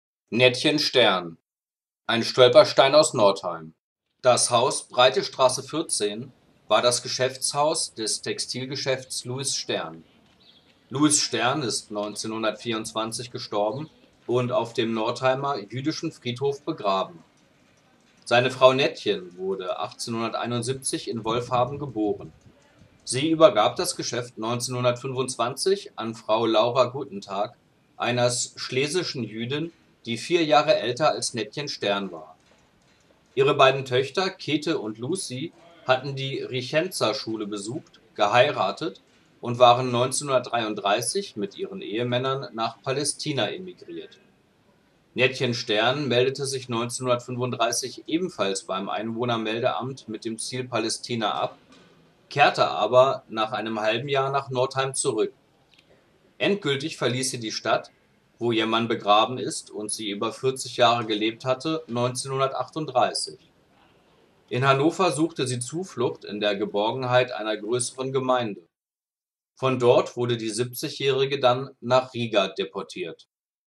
🔴 NETTCHEN STERN • Breitestraße 14 • STOLPERSTEIN für NORTHEIM - EIN KUNSTPROJEKT FÜR EUROPA ...
"Stolpersteine. Ein Kunstprojekt für Europa." - So nennt Herr Gunter Demnig sein Projekt für den Frieden in der Welt! ... Ich bin ein Freund der Stolpersteine, weil ich dieses zufällige erinnern weniger aufdringlich finde als mache monumentalen Gedächtnisstützen, bei denen ich ab und zu eher den Eindruck habe, dass es nicht um das Mahnen geht, sondern um die Selbstdarstellung von Stifter und Architekten. Der dezentrale Moment bei den Stolpersteinen ist meiner Ansicht nach auch besser als geballte Mahnung an einem Ort - so hat man den Eindruck, dass man alles nie erfassen, begreifen und verstehen kann. Es scheint daher um so wichtiger, dass Verfolgung und Krieg nie wieder geschehen.
Bitte TEILEN Sie diesen Film in Ihren sozialen Netzwerken! - DANKE! ...
Herr Hans Harer betreut die Stolpersteine in Northeim. Bei ihm können Sie leicht weitere Informationen erhalten. Ich empfehle eine Kontaktaufnahme. Oft werden in verschiedenen Städten, meist am 27. Januar, oder am 9. November, Gedenkveranstaltungen durchgeführt. Hier wäre Herr Harer der richtige Ansprechpartner! ...
Finden Sie die Stolpersteine als mittel der Mahnung und des Gedenkens in unserer Nachbarschaft gut? ... Werden die Gedenksteine der Würde der Opfer gerecht? ... Ihre Meinung interessiert mich! ... Bitte SCHREIBEN Sie Ihre Meinung dazu in die KOMMENTARE unter den Film... Vielen herzlichen Dank! ...